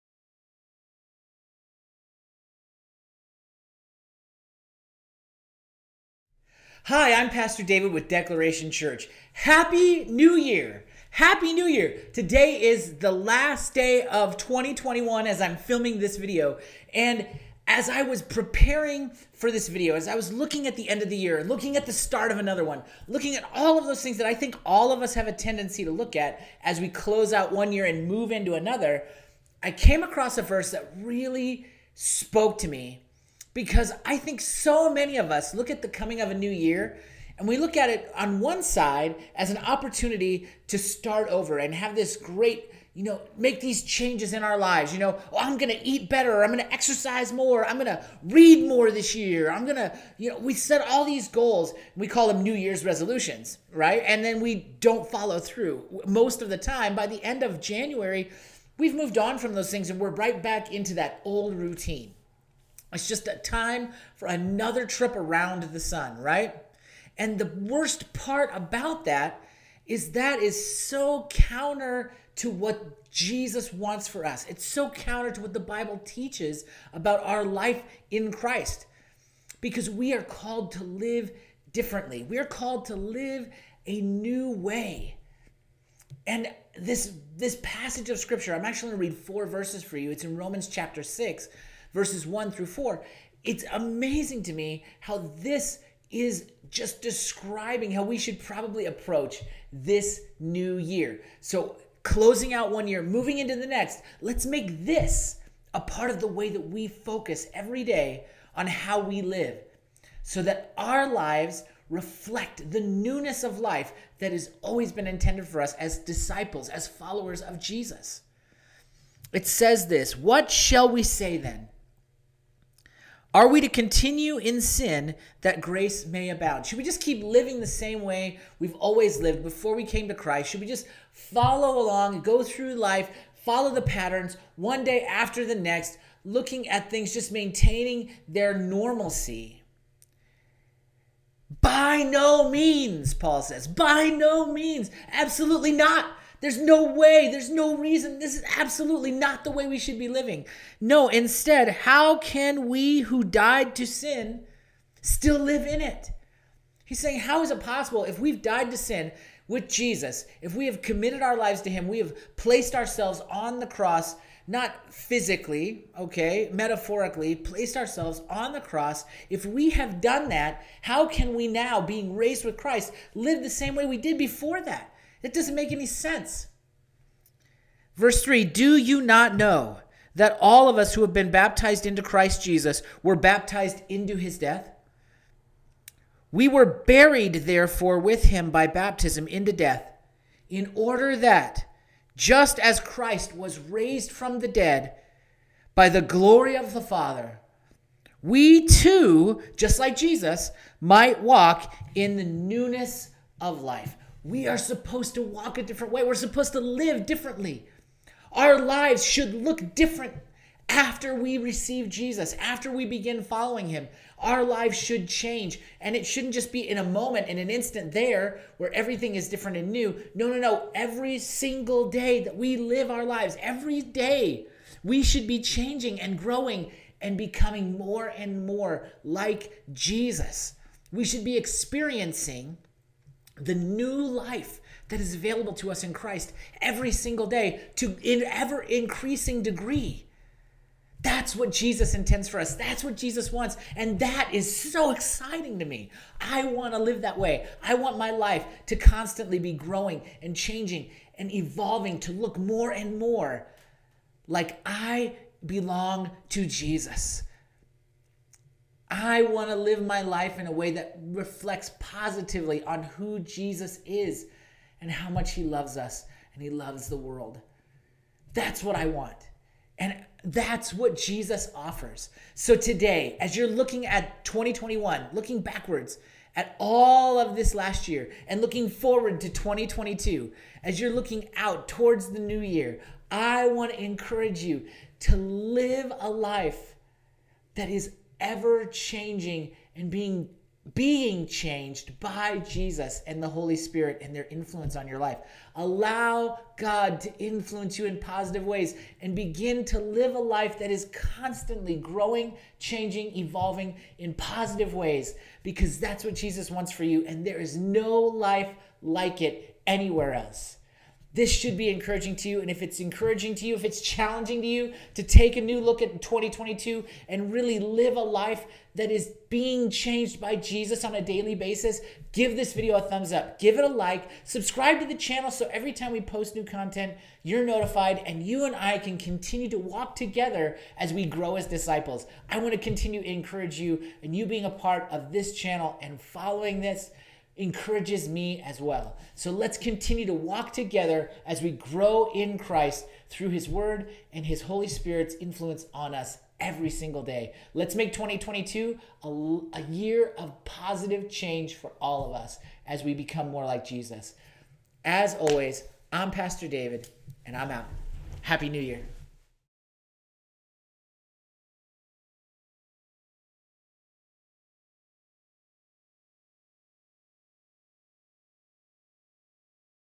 6- Minute Bible Study: Romans 6:4
One year closes, and another begins.  Other than the last digit of the year on the calendar, is there anything else that should change?